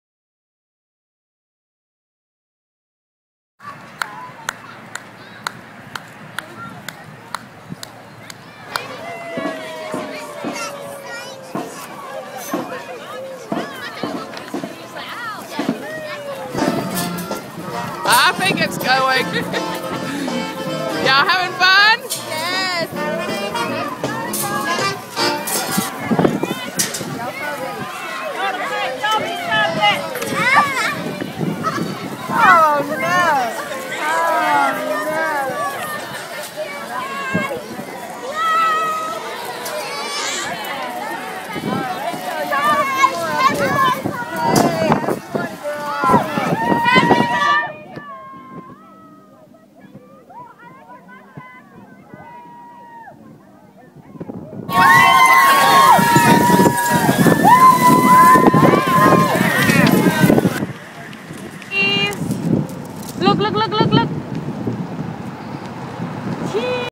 Biloxi Children's Walking Parade
Mardi Gras, Monday morning parade for children.